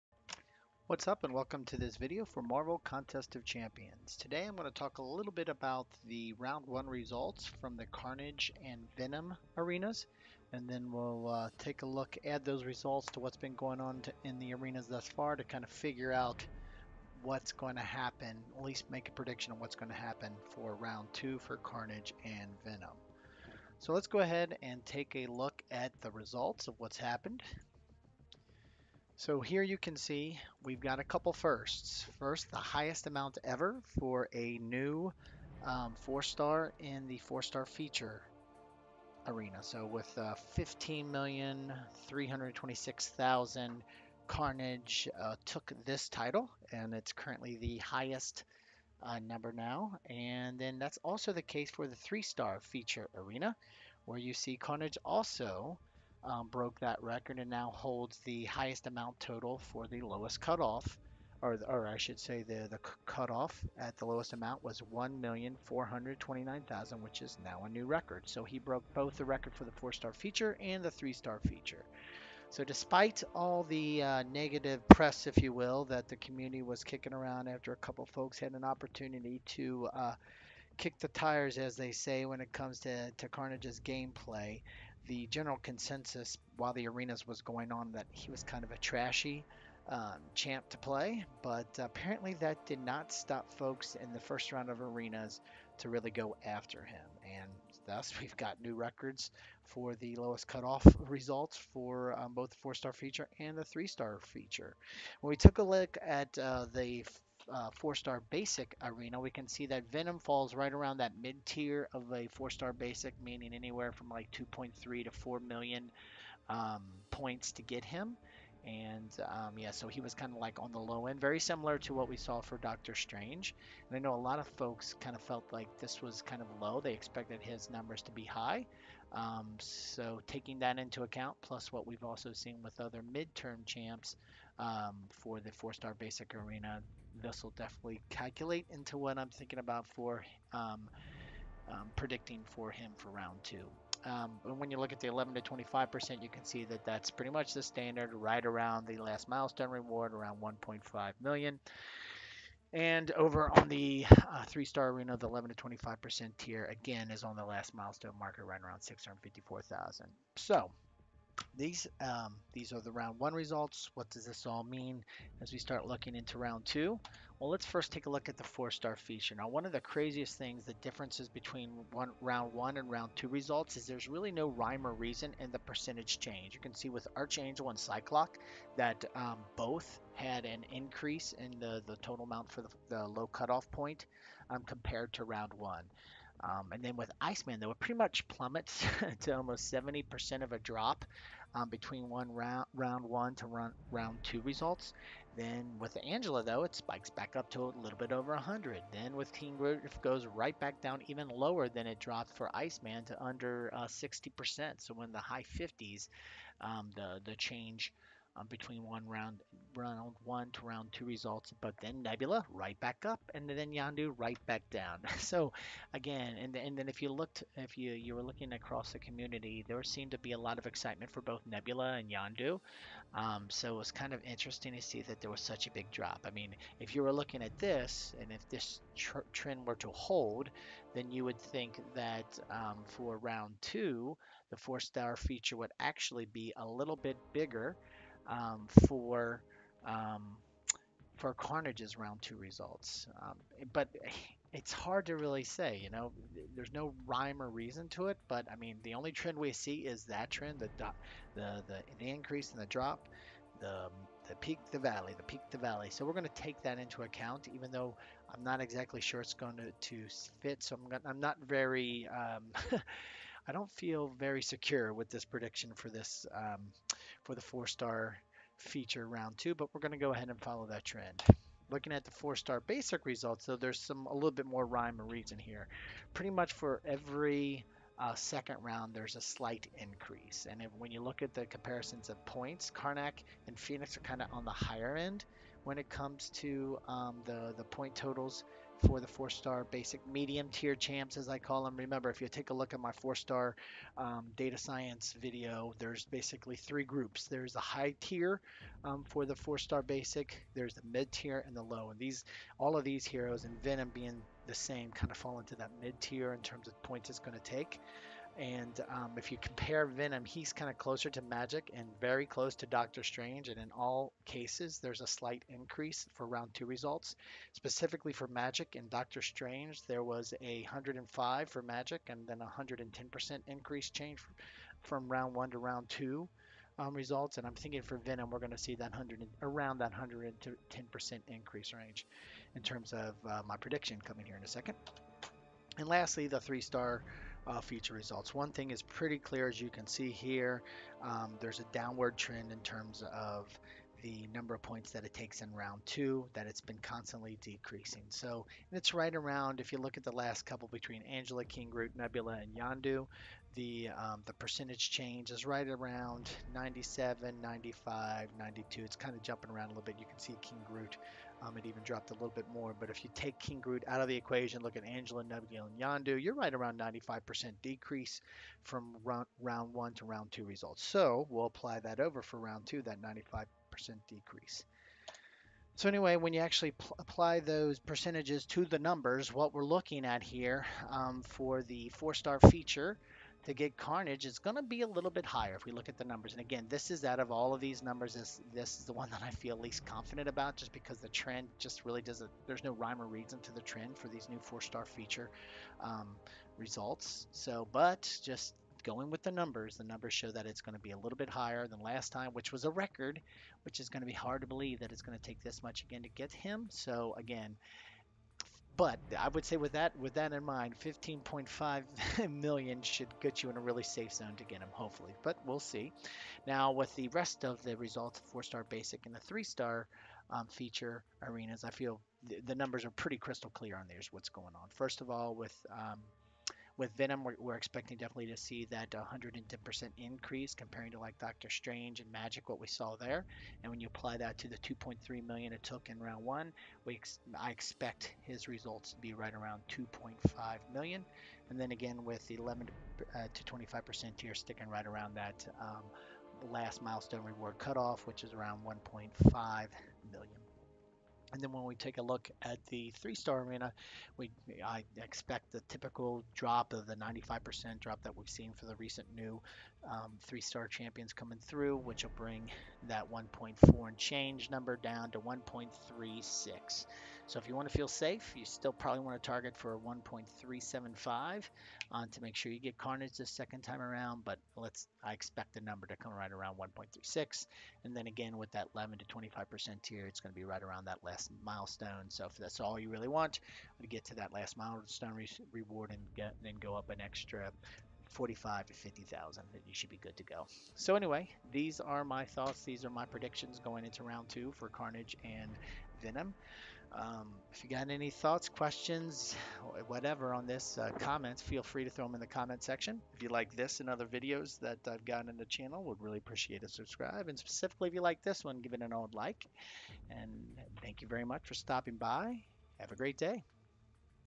Marvel Contest of Champions Arena Cutoff - Carnage and Venom Results & Predictions
This video provides the latest arena round 1 cutoff updates for Carnage and Venom in Marvel Contest of Champions.  I then apply a little data science magic to predict round 2 results.